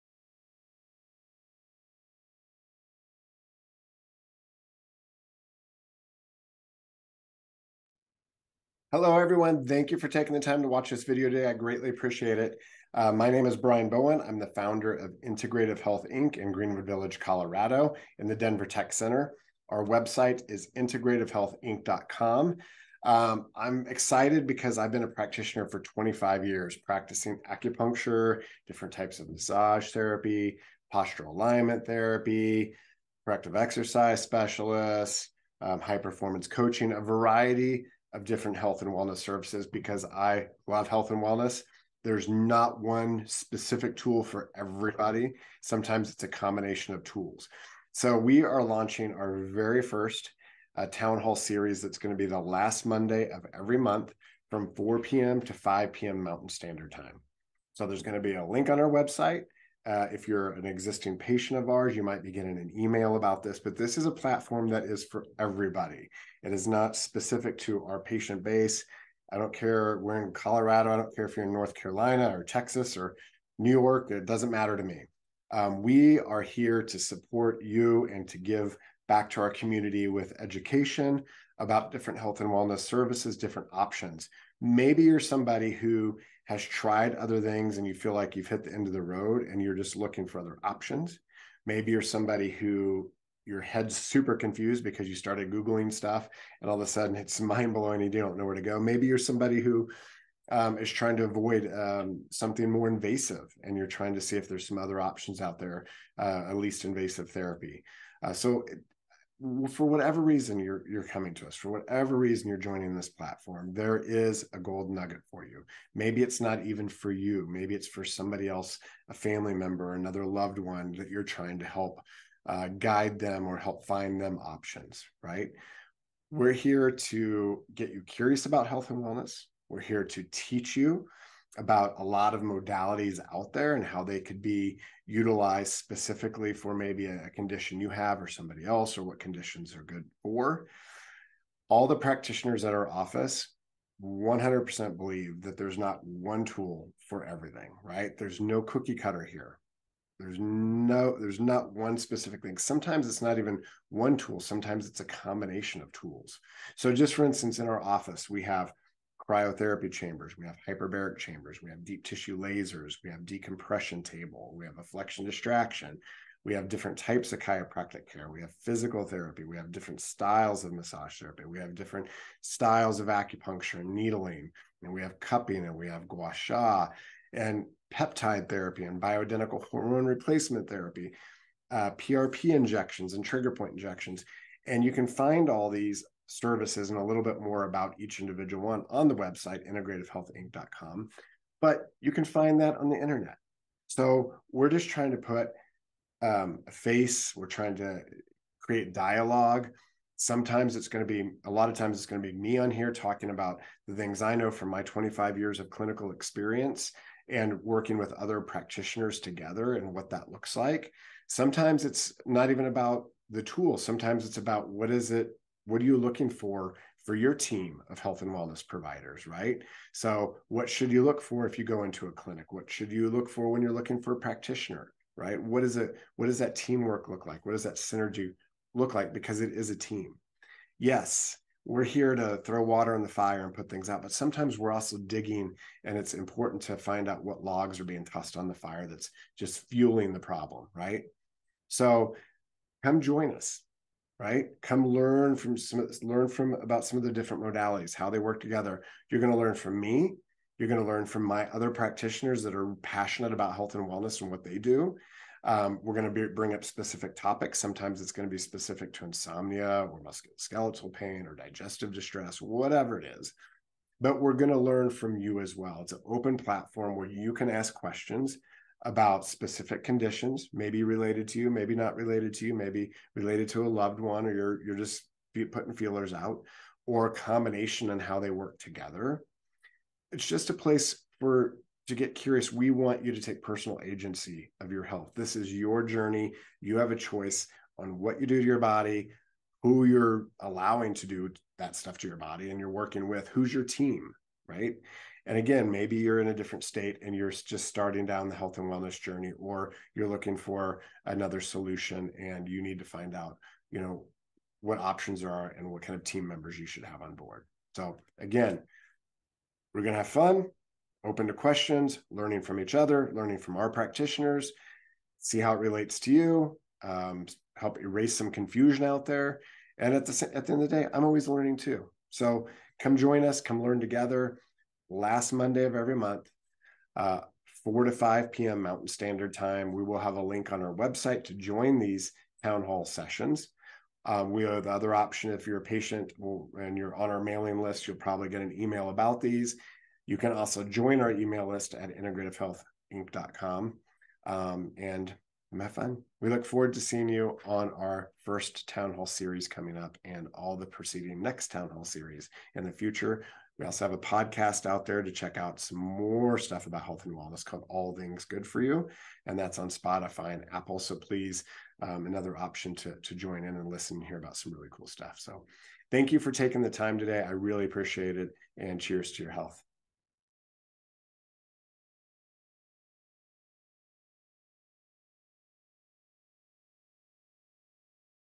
IH Open Forum / Town Hall Series
Be the first to know about new services or updates at Integrative Health, Inc. Get informed about a hot new health topic each month and come to each forum with your own questions to be answered by Brian Bowen, LAc, PAS, LMT, owner, founder and therapist of Integrative Health, Inc.

Integrative Health, Inc's Monthly Forum will be held online the last Monday of each month at 4pm MST.